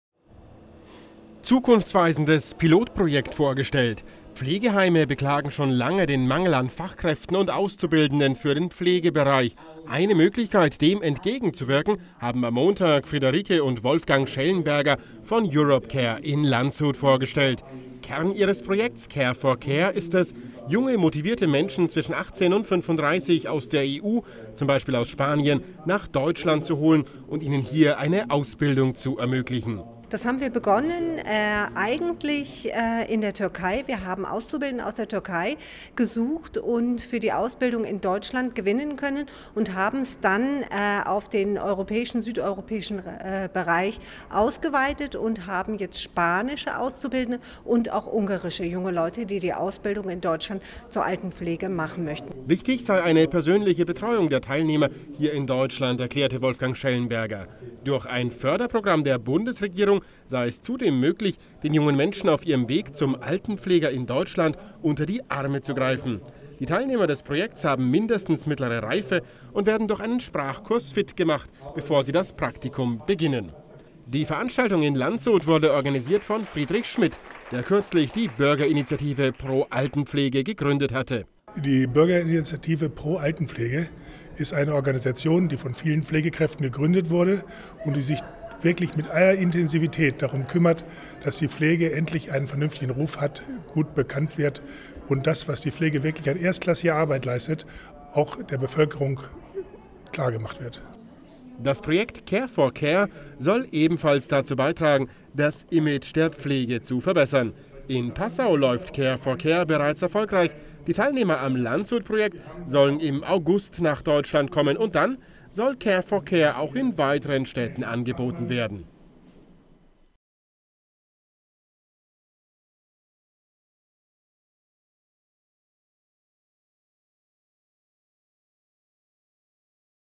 Zukunftsprojekt für die Pflege
"Care For Care" heißt ein neues, in die Zukunft weisendes Projekt für die Pflege. Initiiert von Friederike und Wolfgang Schellenberger von Europcare sollen Pflegekräfte aus anderen europäischen Ländern für Deutschland angeworben werden. Ziel: Dem Fachkräftemangel entschieden entgegen treten - zum Wohle der derer, die nach Deutschland kommen, zum Wohle der Heimbetreiber und vor allem der Pflegebedürftigen.